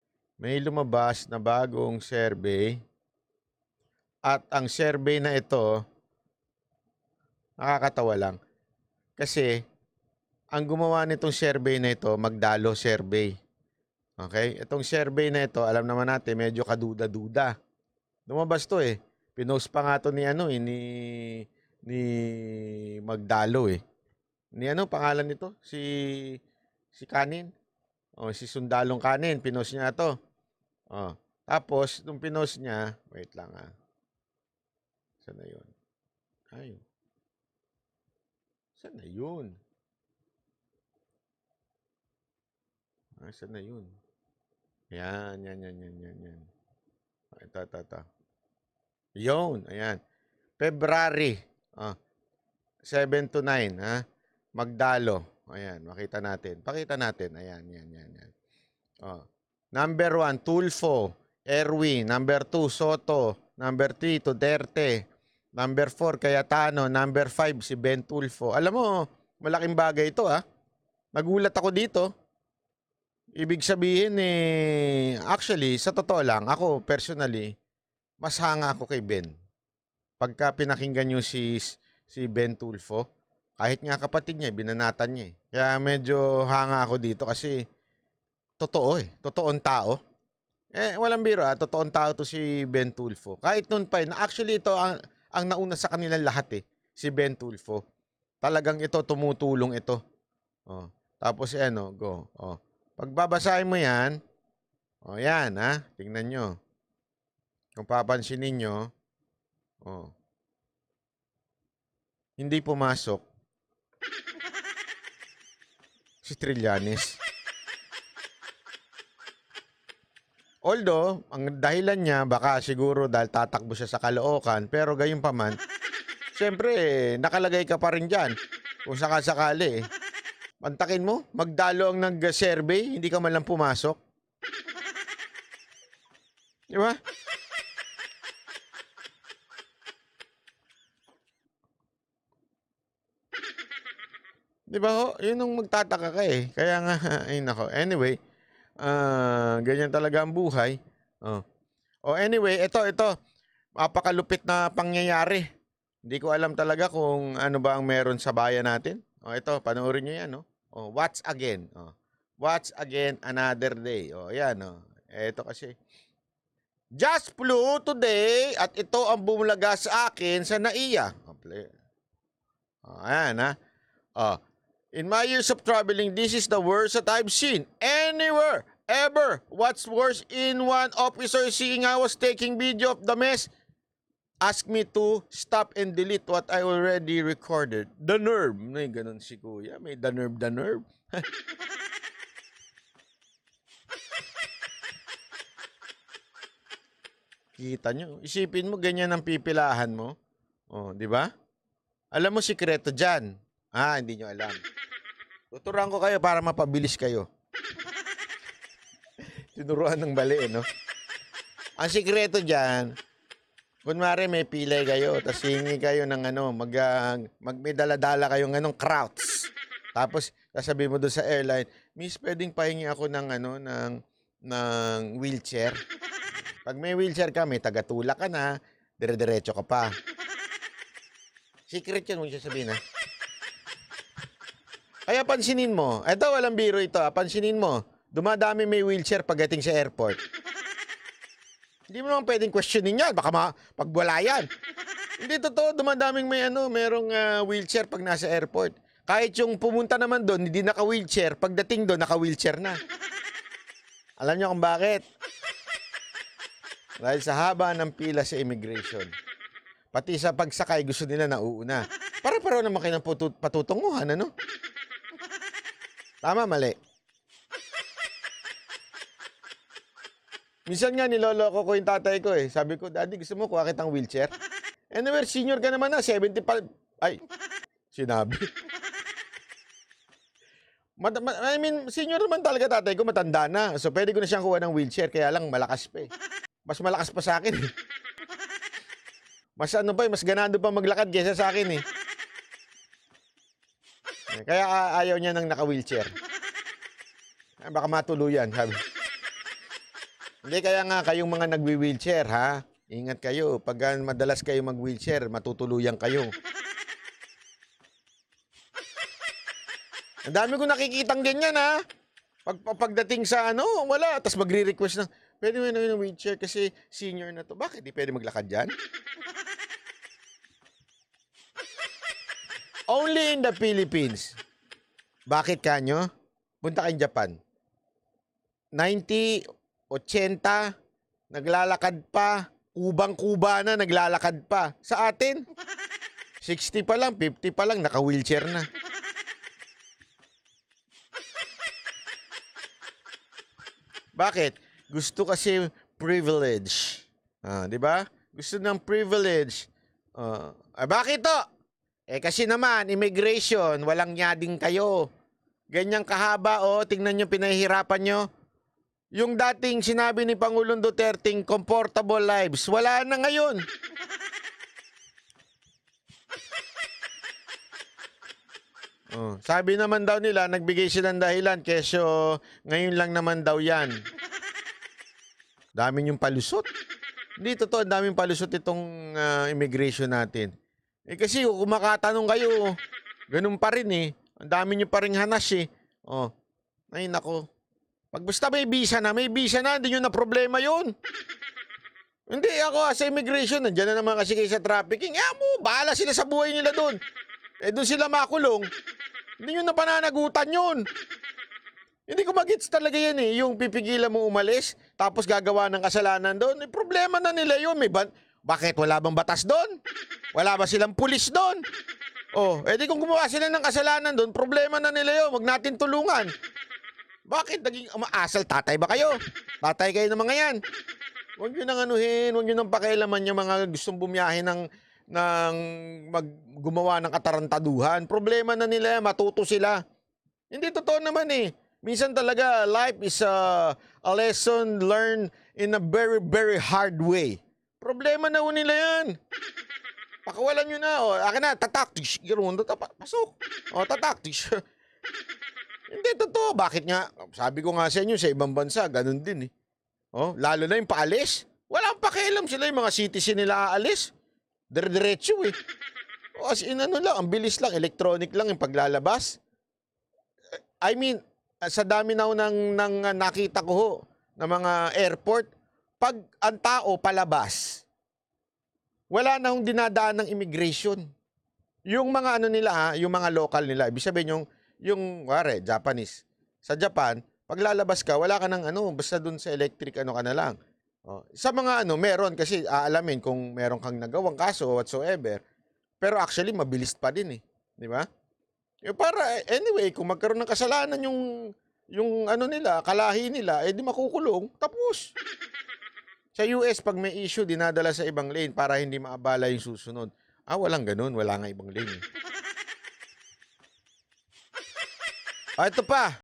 BANAT BY ITO ANG PASOK SA SURVEY EX PRRD TITO SOTTO BEN TULFO AT ERWIN IYAK SI TAMBA AT TRILLANES
Watch for more Banat News Update in the Philippines